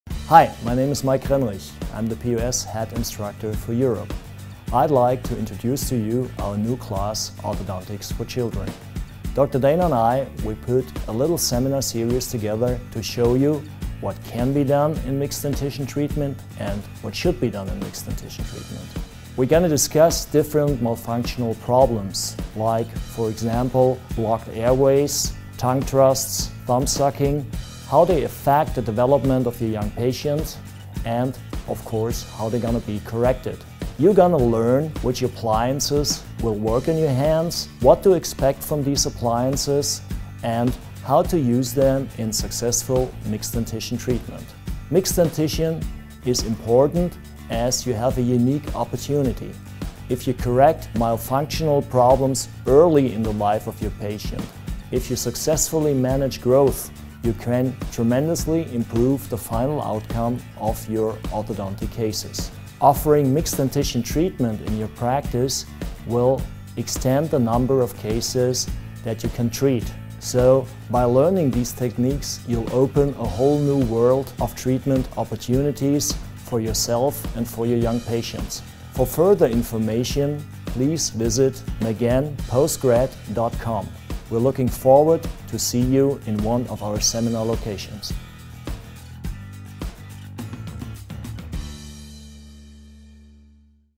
Course Overview: Orthodontics for Children with Dr. Mike Krennrich | Progressive Orthodontics
This course is designed for dentists to be able to provide a complete diagnosis for the children in their practices. There is a total of 6 days covering how to evaluate, diagnose and treatment plan the most common dental and skeletal malocclusion problems found in mixed dentition (e.g. antero-posterior, vertical, transverse, and habit problems) and what appliances to use. Anticipation of potential complications and recognition of problems early will help you achieve excellent results.